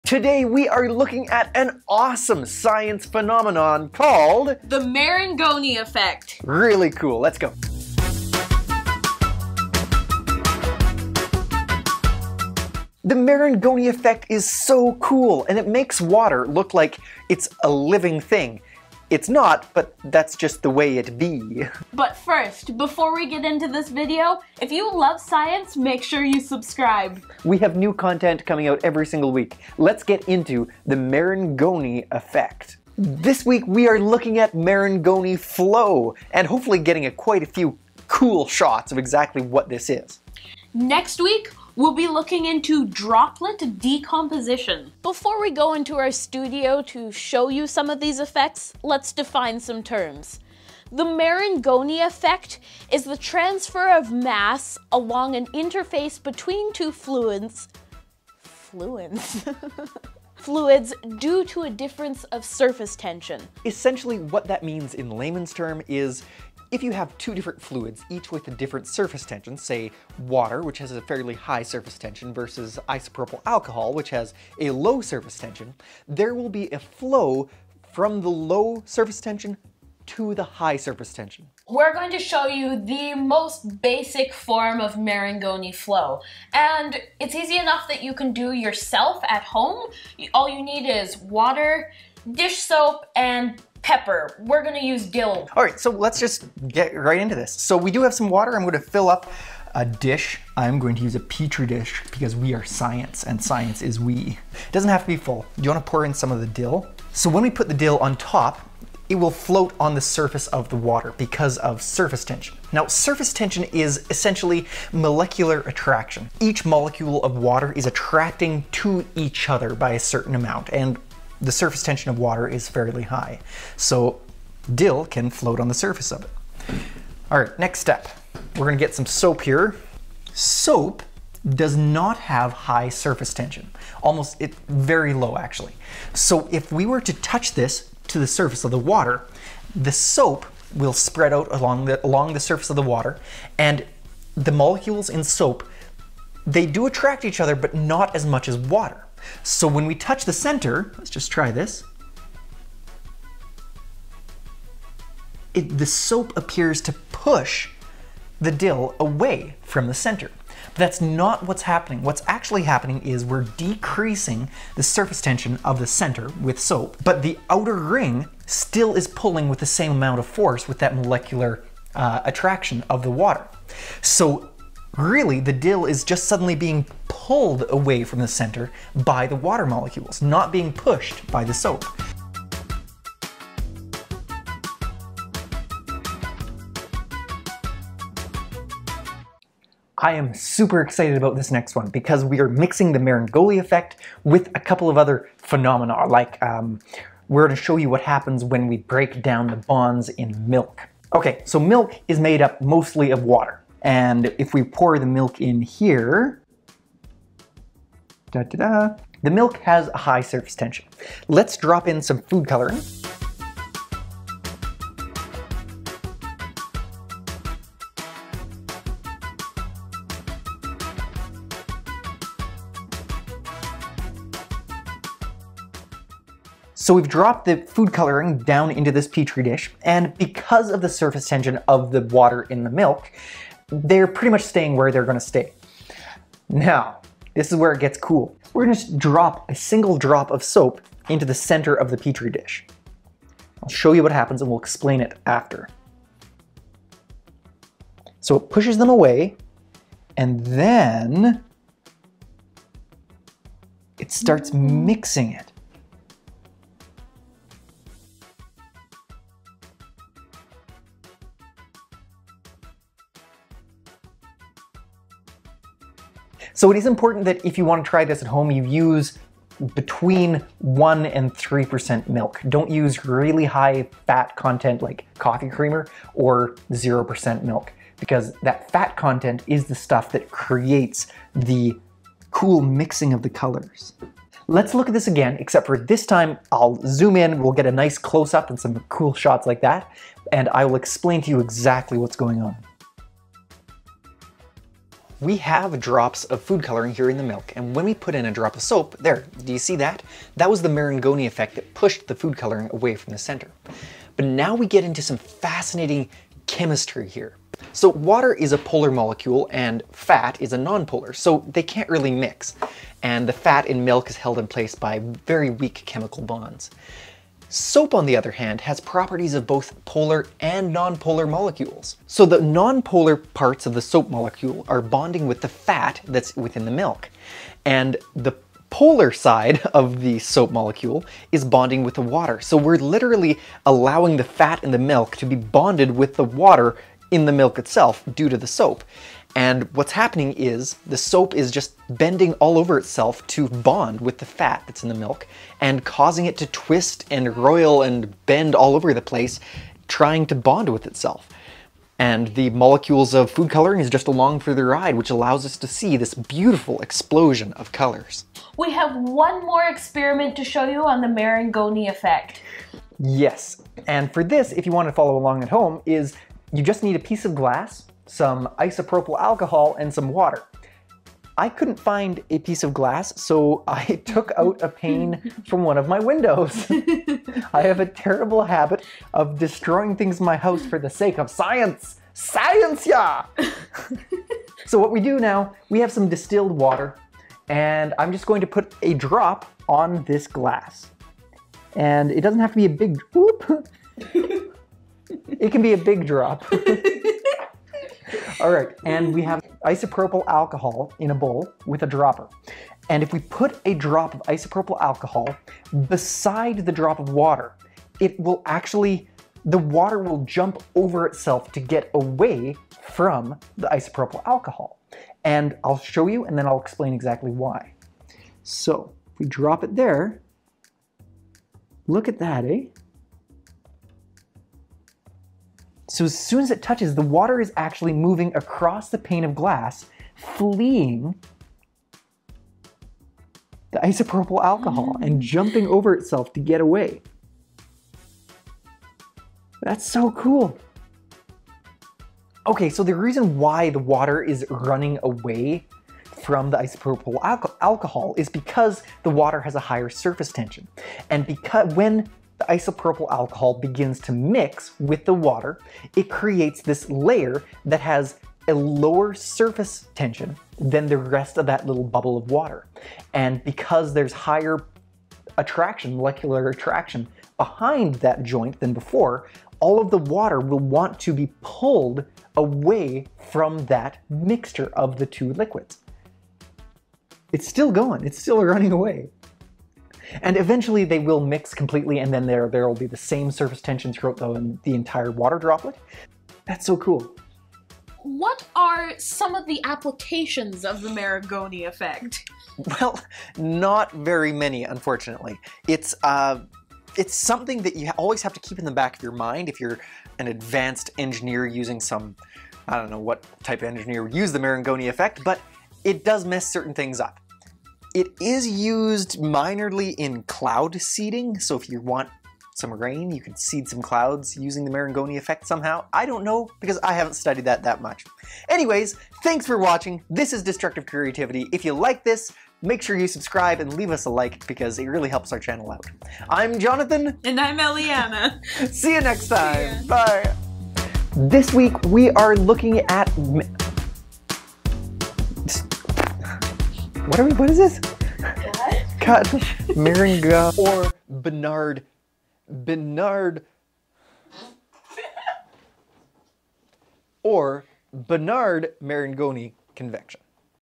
Marangoni effect! Surface tension, magic milk experiment and why it works!
The Marangoni effect is an awesome high school physics phenomenon about surface tension and fluid dynamics that looks like magic!
00:00 Intro to Destructive Creativity
00:45 Defining of terms and explanation
01:20 Experiment with Marangoni Flow and pepper. Molecular attraction.
03:10 Magic milk with food coloring, polar and nonpolar molecules.
05:58 Alcohol chasing water using the marangoni effect.
08:09 Application of the Marangoni effect.
08:40 Conclusion for Destructive Creativity

Today we take on three different experiments regarding Marangoni Flow, the classic soap and pepper flakes, magic milk, and a cool one where alcohol chases water around a sheet of glass! So cool!
Destructive Creativity and Jonathan Allers are dedicated to providing safe and fun science experiments and facts for all ages. Please science responsibly!